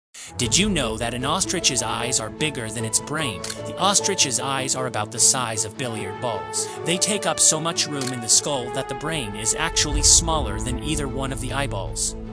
Ostrich’s Eyes
Did you know that An ostrich’s eyes are bigger than its brain? The ostrich’s eyes are about the size of billiard balls. They take up so much room in the skull that the brain is actually smaller than either one of the eyeballs.